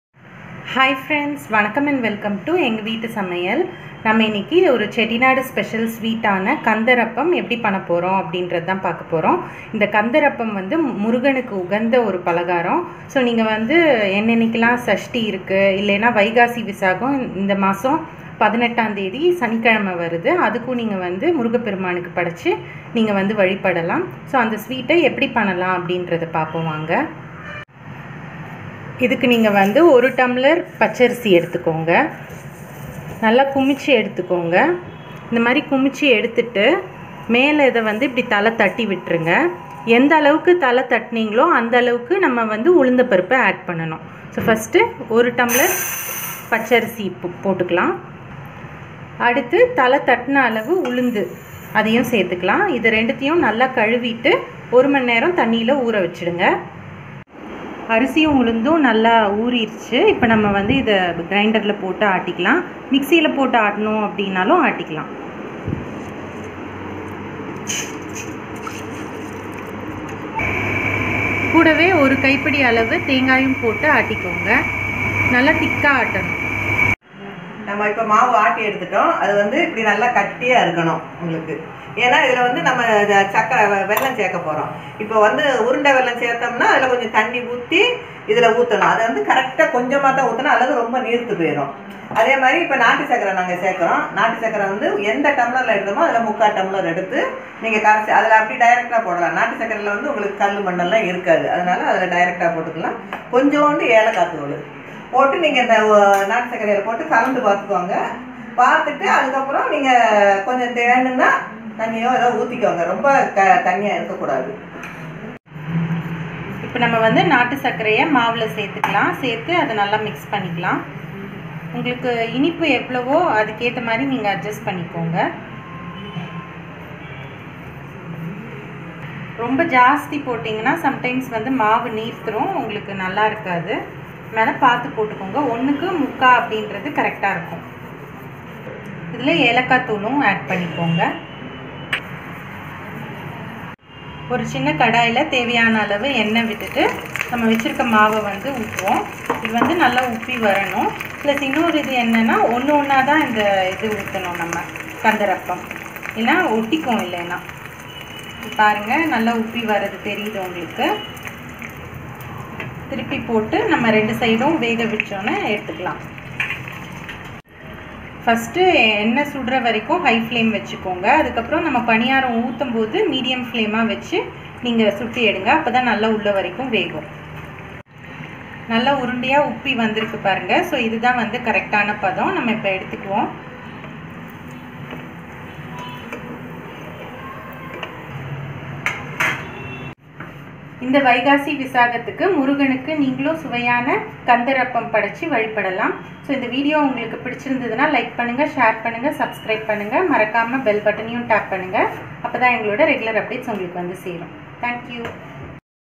செட்டிநாடு கந்தரப்பம் | Kandarappam | Vaikasi Visakam 2019 Special | Kandarappam Recipe
Vaikasi Visakam 2019 Special(Vaikasi Visakam Viratham Special Kandarappam/Vaikasi Visakam Vratham Special Kandarappam)

செட்டிநாடு கந்தரப்பம்  (Chettinad Kandarappam Recipe / Chettinad

Sweet Recipes in Tamil). In this video, we have shown the traditional
method to prepare Chettinadu special sweet " Kandarappam" which is a favorite dish for Lord Murugan.